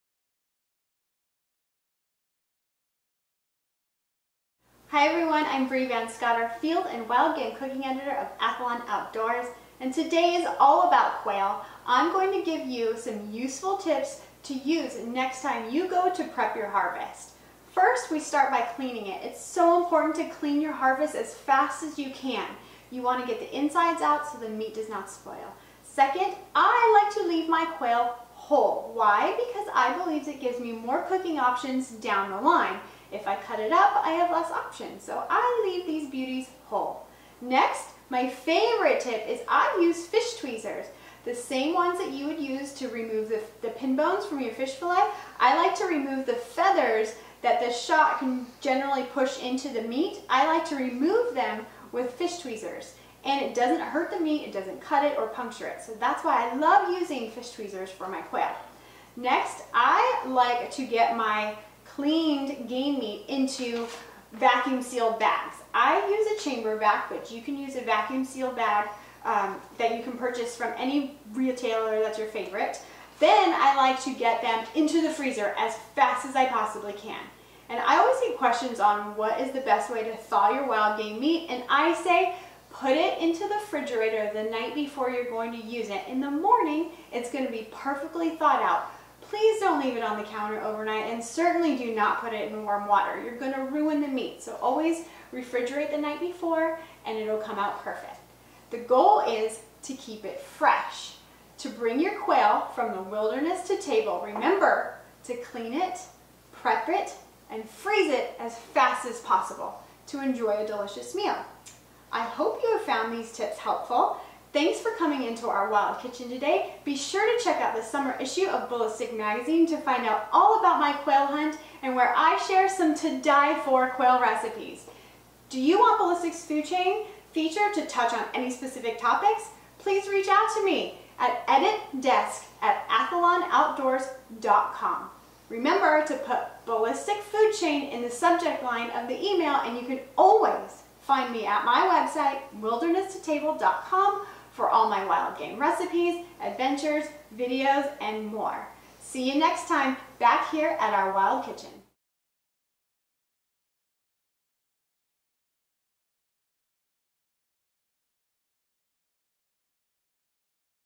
Wild Kitchen, Episode 1: Quail Prep with Bri Van Scotter
Welcome to "Wild Kitchen" with Athlon Outdoors author Bri Van Scotter. Known for her platform "Wilderness to Table," Bri will take you through the steps of not only hunting your own game, but preparing it and plating it. In Episode No. 1, Bri discusses the right way to go about quail prep.